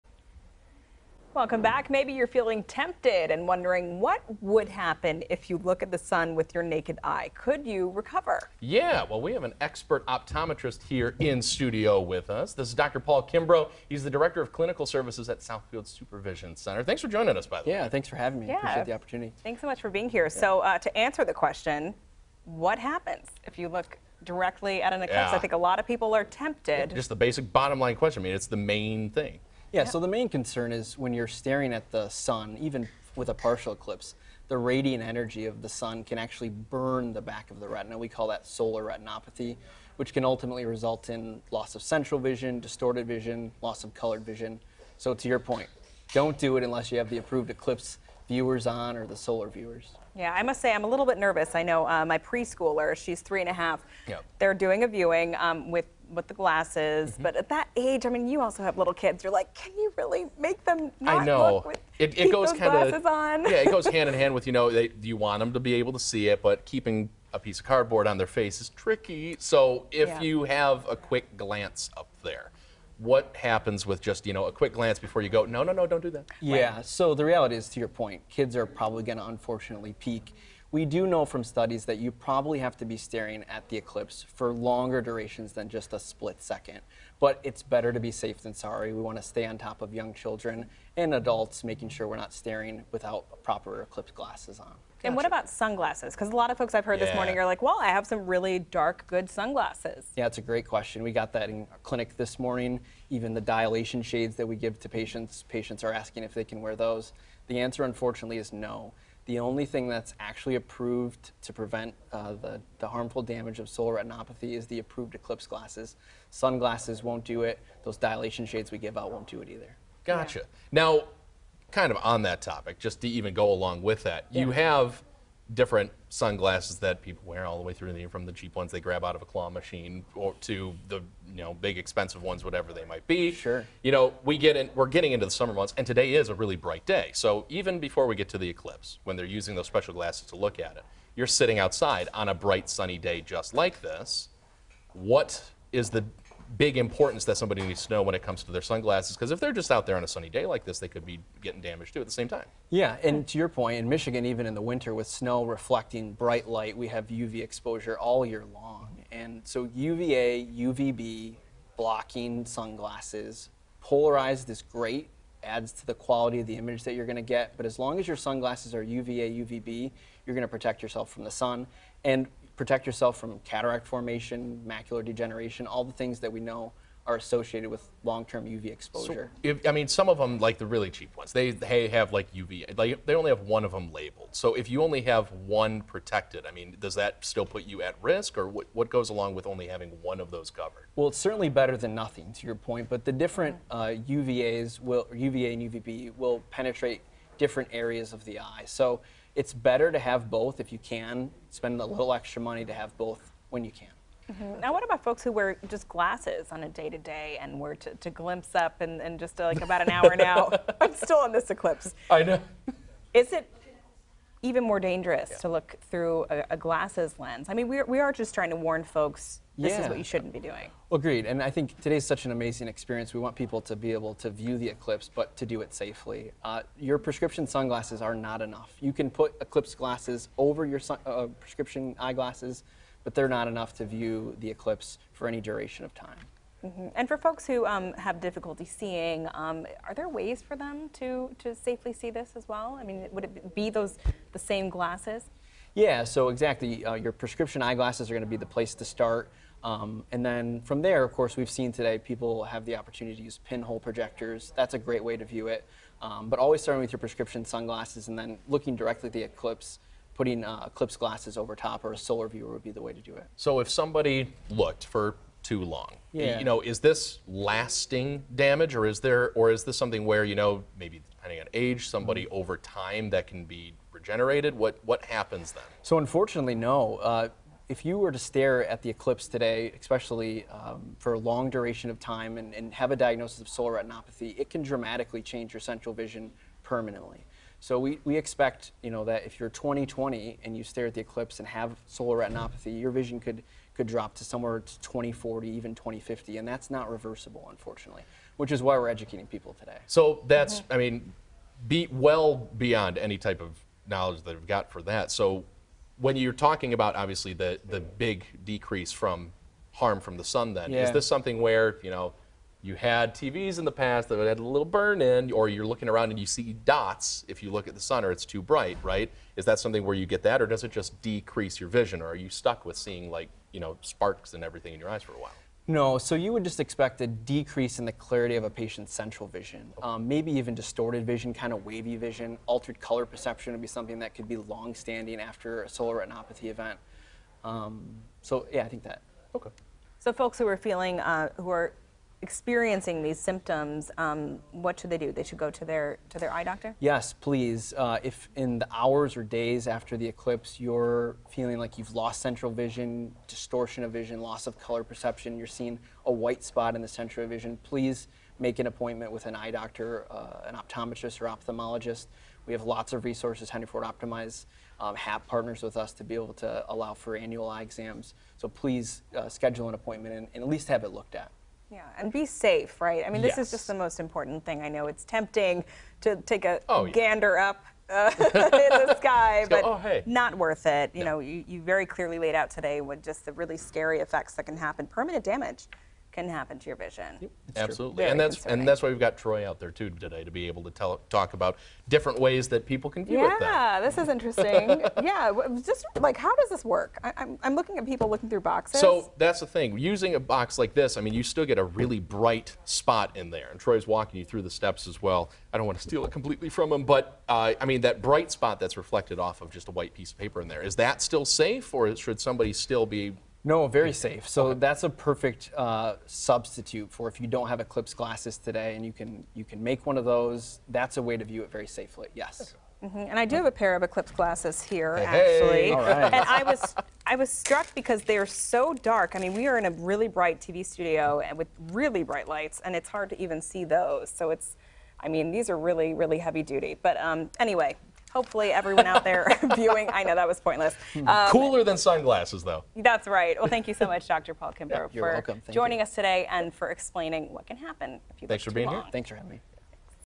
How to protect your eyes during a solar eclipse
Paul Kimbro, the director of clinical services at Southfield Super Vision Center with OptimEyes, shares more on protecting your eyes during a solar eclipse.